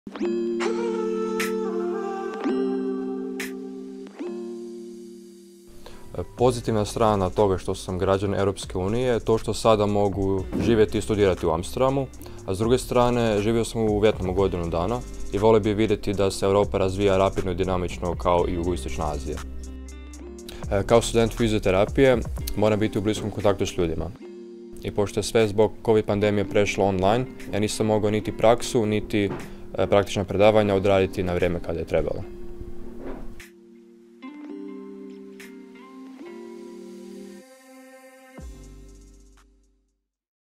In Hum, Croatia, student explains aspects of Europe and how the pandemic affected him - Short Story
Episode 6 - HUM, Istria: In this sixth film Matko Kmezić, a young physiotherapist student from Croatia studying in The Netherlands explains his views on Europe and how the pandemic affected him.
This is the 6th of the 8 short 1-minute films I directed for the Ministry of Demographics and Youth of Croatia (@
These films were made for the 'Conference of the Future of Europe' asking 2 questions, 'what are your thoughts about the future of Europe and how did the pandemic affect you'.
I interviewed either Croats who lived abroad and returned to Croatia, or foreigners currently living in Croatia.
In the next episodes I will be interviewing, Steve Hunt and Tanya Bendiš . Also checkout the previous episodes in my feed, where I interview Jan de Jong, Giorgio Clai from Clai winery, Iva Prgomet, José Rodrigues and Joseph Le Corre.
Share freely!